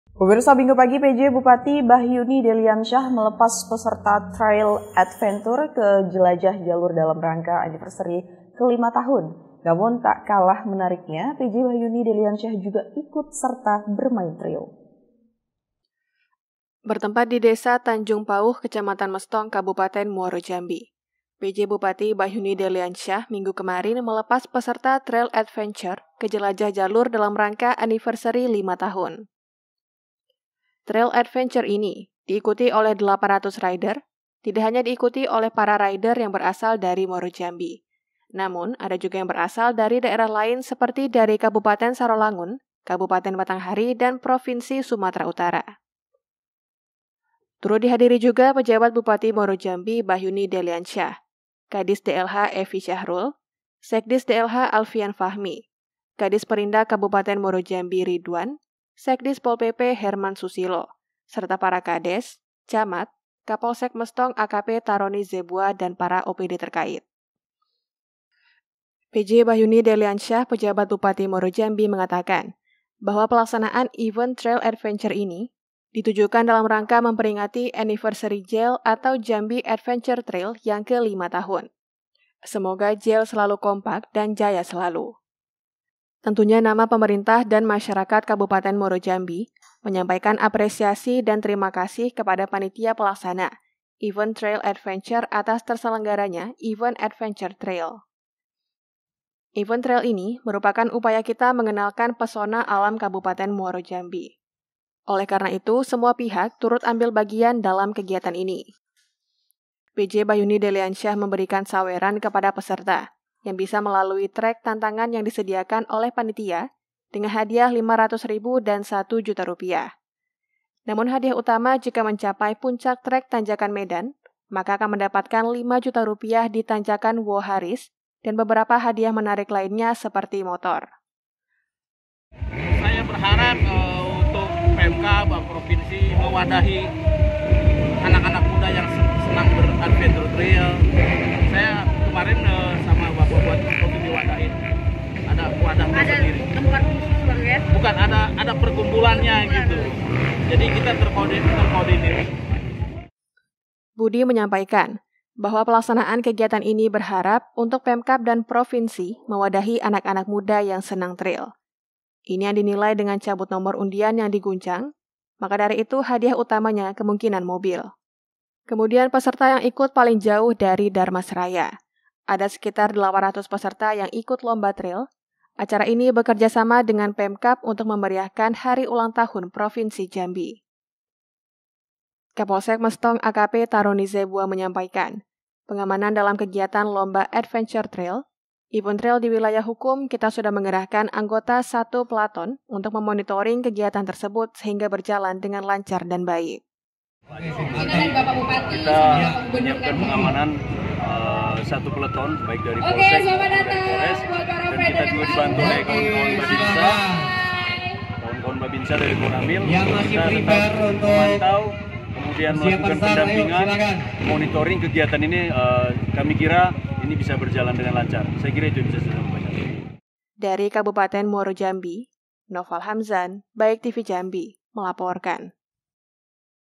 PJ BACHYUNI DELIANSYAH SECARA RESMI LEPAS PESERTA TRAIL ADVENTURE TAHUN 2023
BNEWS EDISI 21 FEBRUARI 2023